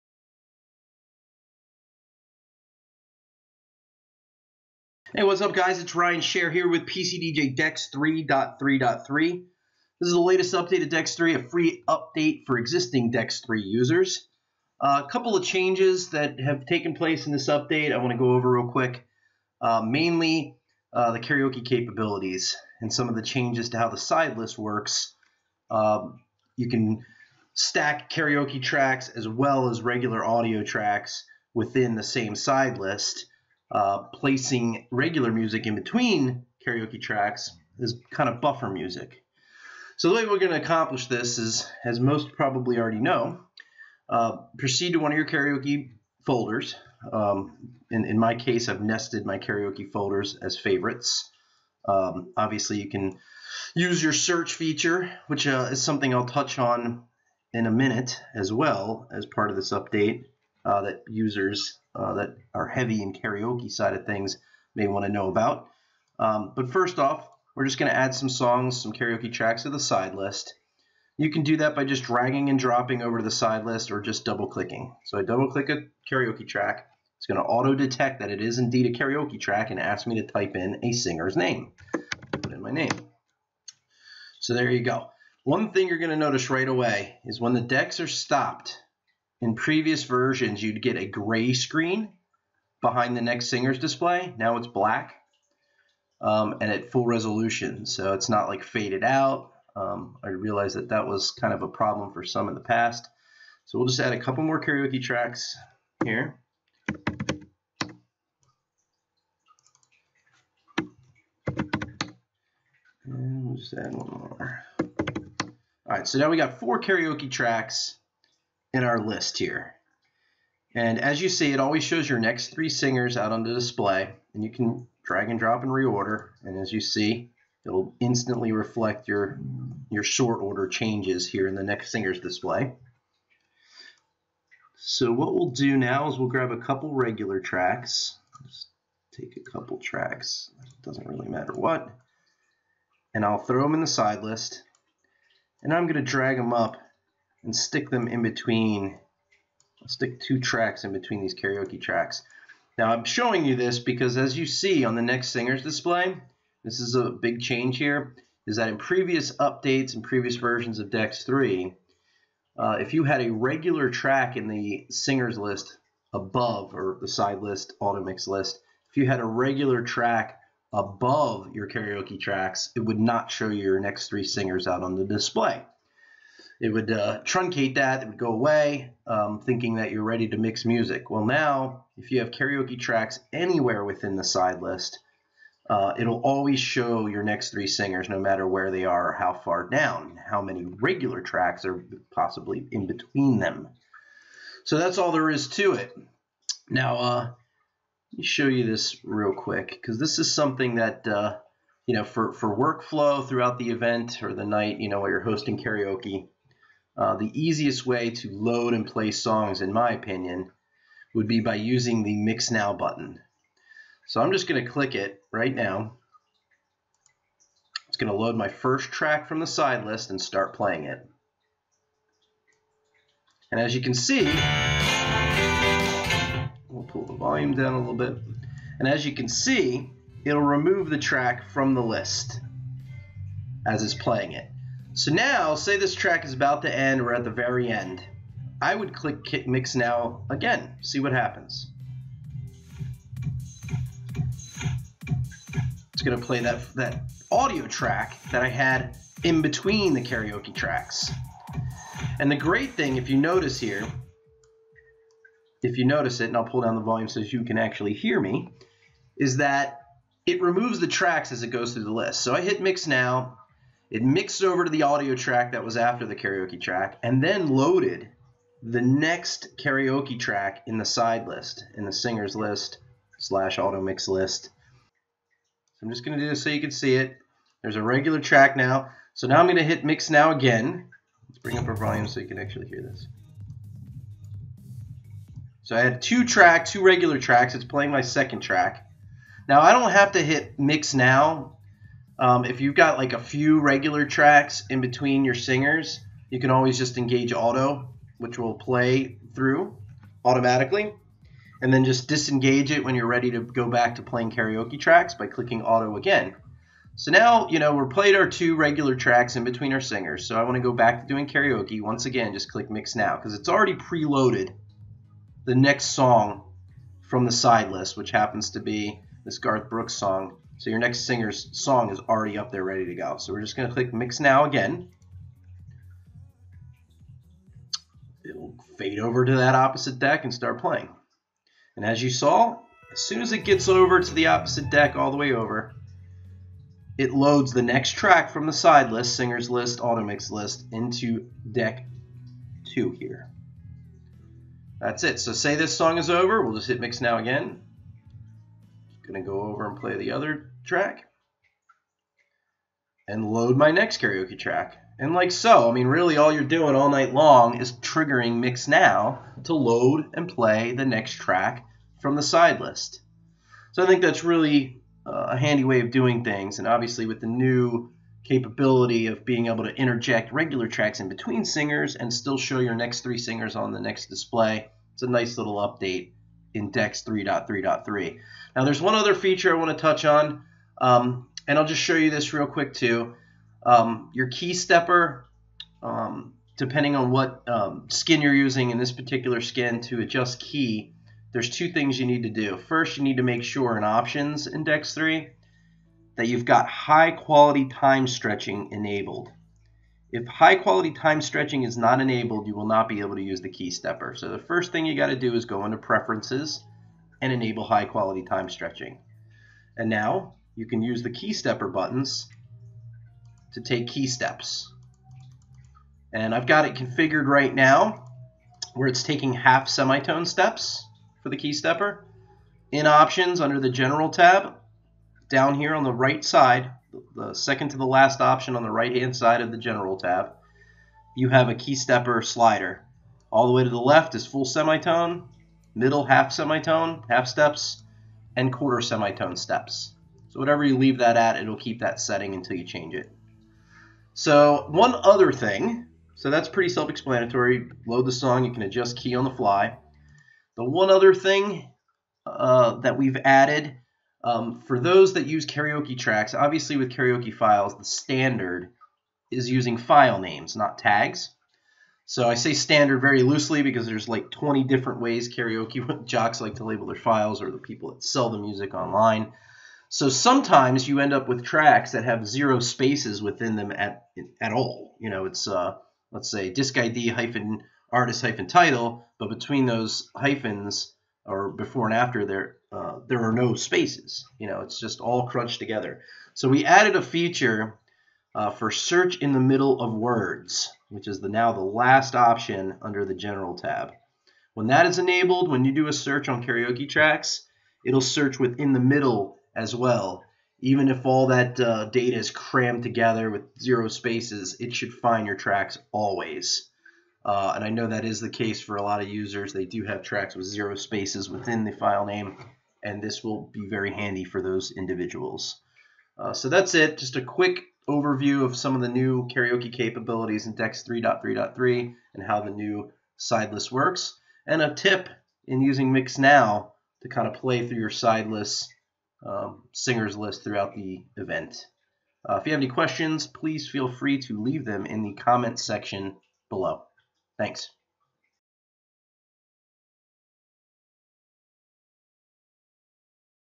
PCDJ DEX 3 | Karaoke Features Now Included In 3.3.3
In this tutorial video overview we show you the new karaoke features included with our latest DJ software updates - version 3.3.3.

We'll show you how the new singers list features work, how to adjust key on a track and touch on a new search feature for searching karaoke files.